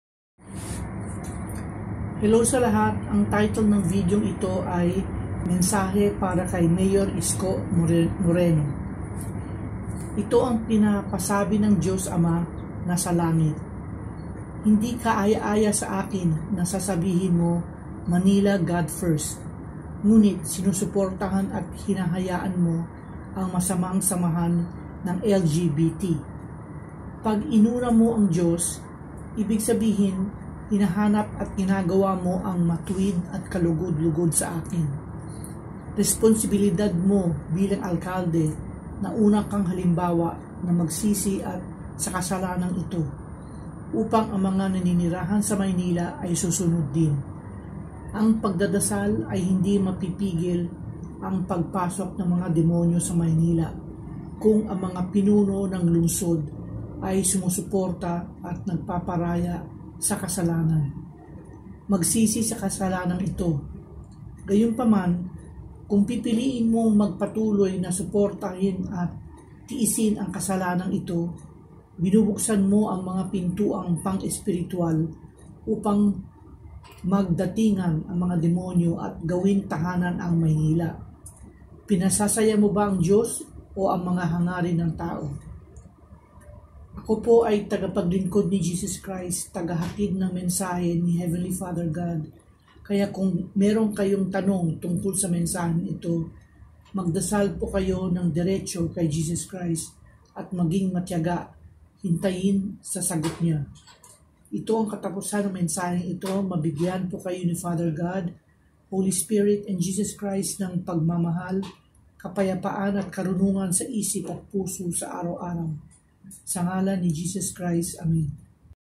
Mensahe para kay Mayor Isko Moreno
Hello sa lahat ang title ng video eto ay:   Mensahe para kay Mayor Isko Moreno

Ito ang pina-pasabi ng Diyos Ama nasa Langit:  Hindi kaaya-aya sa Akin na sasabihin mo  "Manila, God First!" Ngunit sinusuportahan at hina-hayaan mo ang masamang samahan ng LGBT.  Pag inuna mo ang Diyos, ibig sabihin hinahanap at ginagawa mo ang Matuwid at kalugod-lugod sa Akin.  Responsibilidad mo bilang Alkalde na una kang halimbawa na magsisi sa kasalanang ito, upang ang mga naninirahan sa Maynila ay susunod din.  Ang pagda-dasal ay hindi mapipigil ang pagpasok ng mga demonyo sa Maynila kung ang mga pinuno ng lungsod ay sumusuporta at nagpaparaya sa kasalanan.  Magsisi sa kasalanang ito!  Gayunpaman, kung pipiliin mong magpatuloy na suportahan at tiisin ang kasalanang ito, binubuksan mo ang mga pintuang pang-espiritwal upang magdatingngan ang mga demonyo at gawin tahanan ang Maynila.  Pinasasaya mo ba ang Diyos o ang mga hangarin ng tao?

Ako po ay tagapag-linkod ni Jesus Christ.  Taga hatid ng mensahe ni Heavenly Father God, kaya kung meron kayong tanong tungkol sa mensaheng ito, magdasal po kayo ng direcho kay Jesus Christ, at maging matyaga hintayin sa sagot Niya.

Ito ang katapusan ng mensahe ito.  Mabigyan po kayo ni Father God, Holy Spirit and Jesus Christ ng Pagmamahal, Kapayapaan at Karunungan sa isip at puso sa araw-araw, sa ngalan ni Jesus Christ, amen.

Facebook:  Kilala Ka Ba Ni JESUS CHRIST?